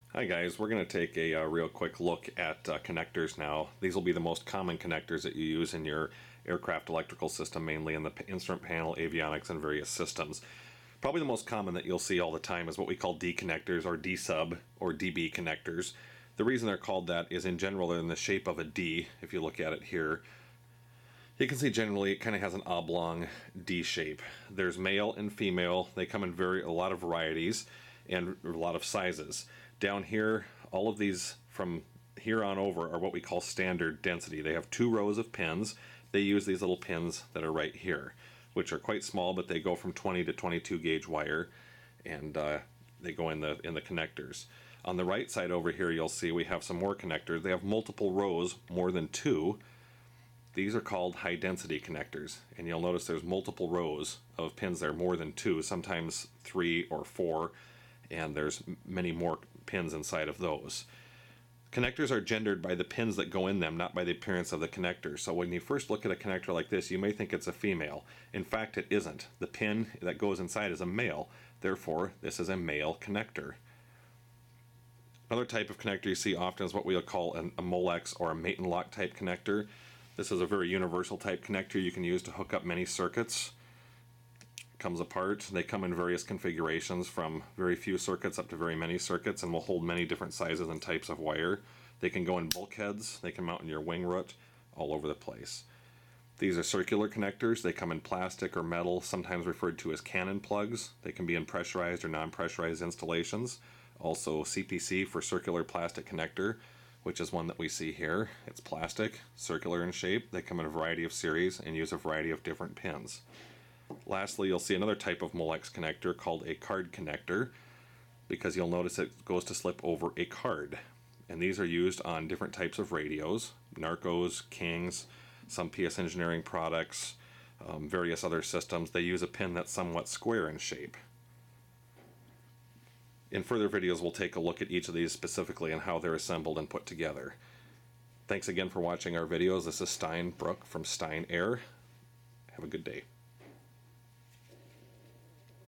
003. Connectors: Basic overview of aircraft Dsub, Molex and Circular Connectors
Very basic look at what a Dsub connector is compared to a molex and others.